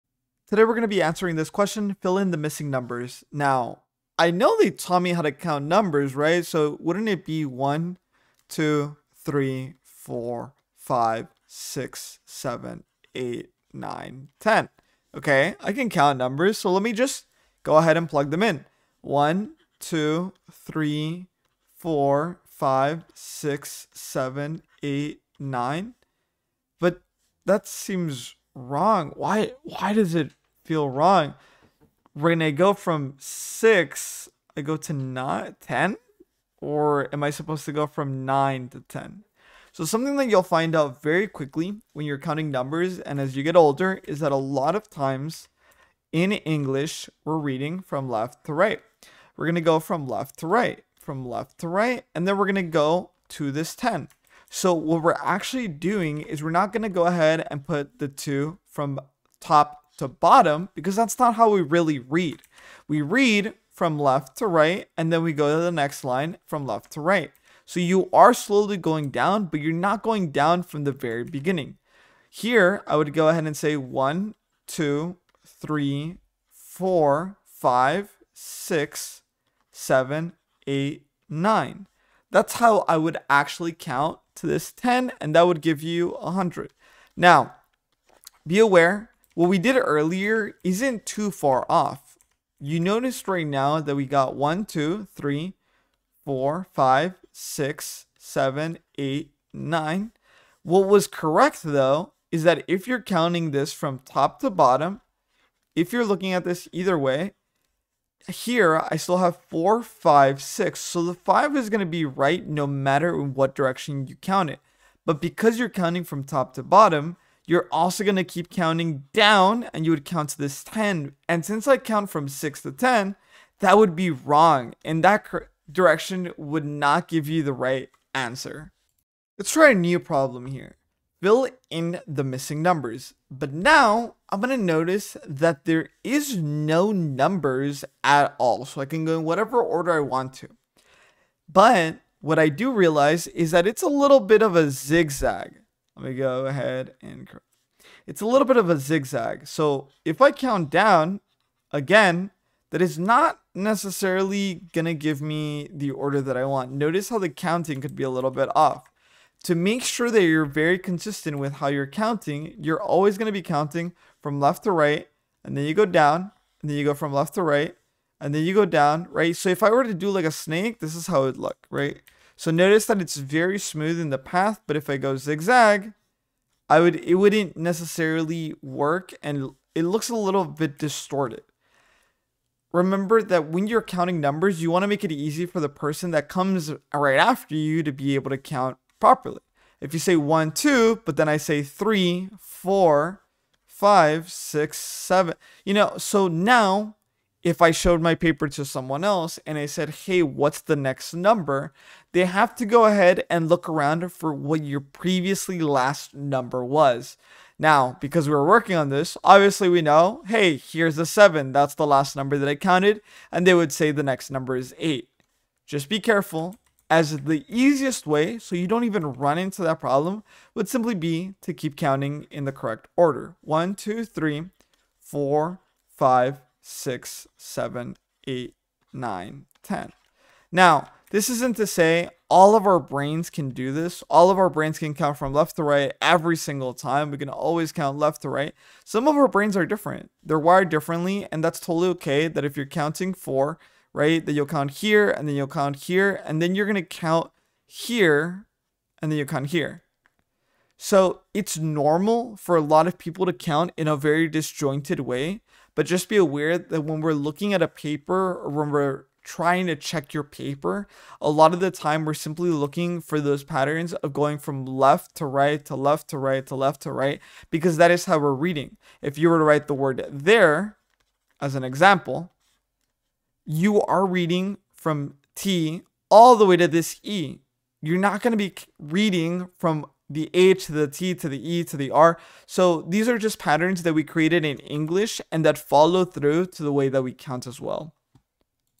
How to Count In Order | Elementary Math
Learning how to count in order is the foundation of math success! In this video, we’ll walk through the basics of counting sequentially, perfect for young learners or anyone wanting to strengthen their elementary math skills. Watch as we use fun visuals, engaging examples, and easy-to-follow explanations to help make counting a breeze.

Whether you're a student, parent, or teacher, this video is designed to make math fun and accessible for everyone.